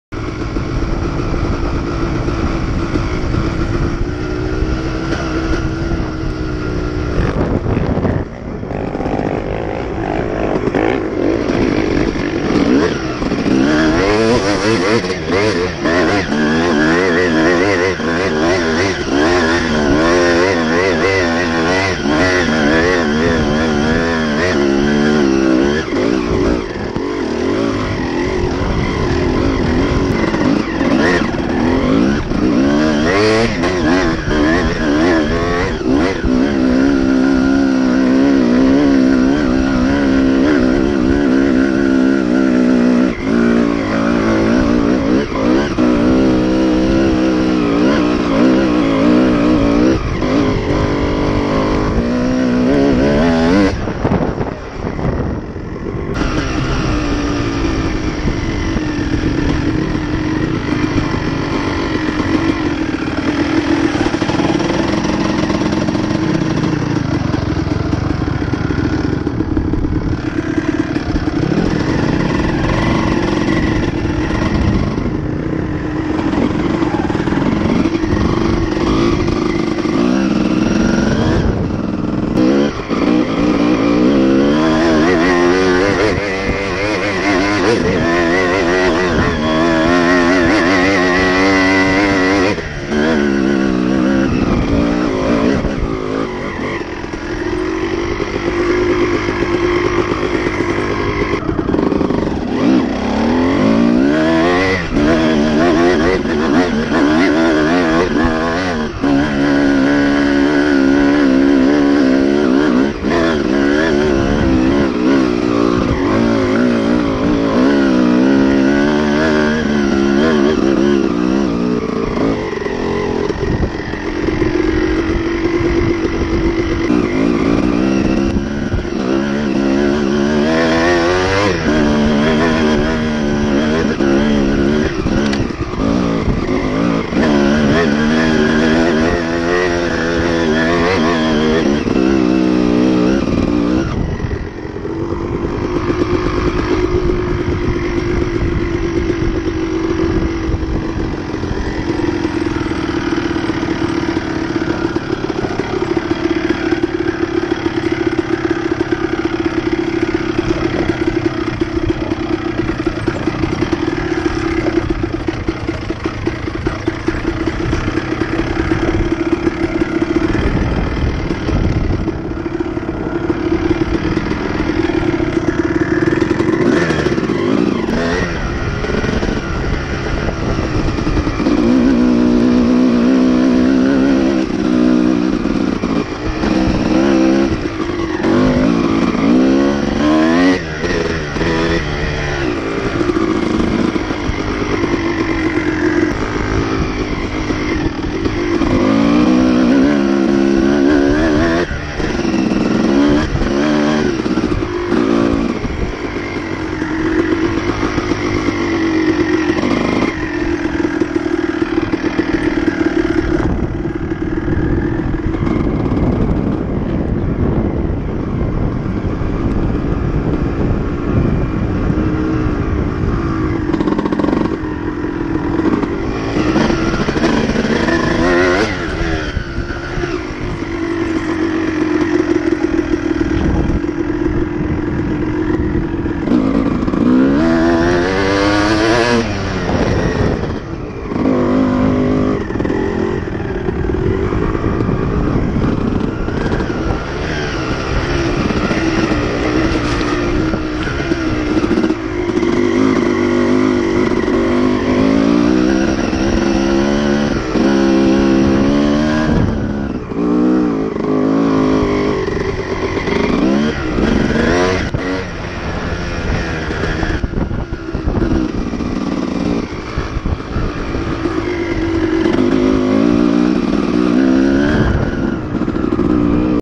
Carnegie 3-12-16
Riding in the mud with Jason